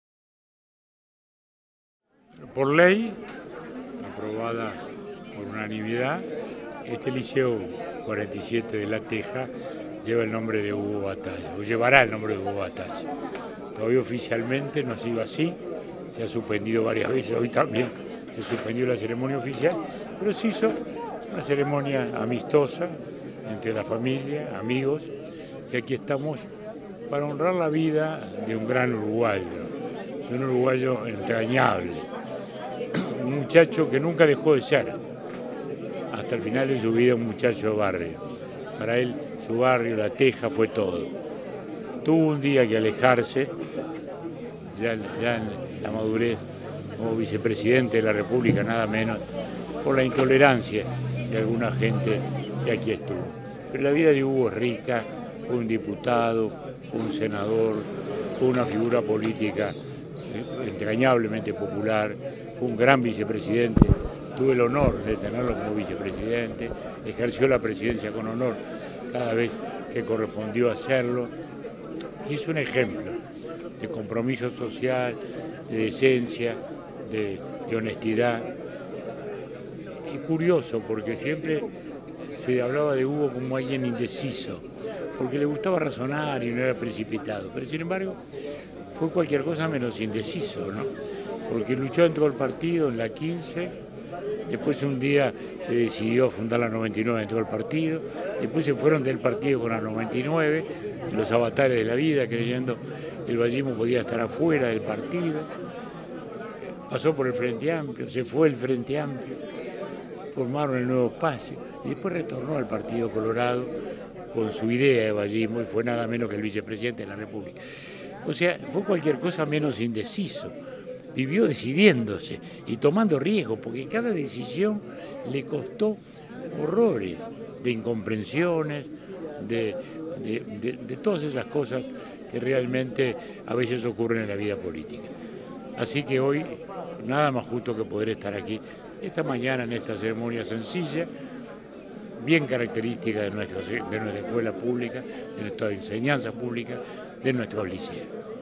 Julio María Sanguinetti en homenaje a Hugo Batalla
Tras un sencillo acto el ex Presidente recuerda la figura de Hugo Batalla y su vida política. El liceo Nº 47 de La Teja fue nombrado recientemente “Hugo Batalla” y en ese marco familiares y amigos se concentraron en este liceo para recordar la trayectoria de Batalla.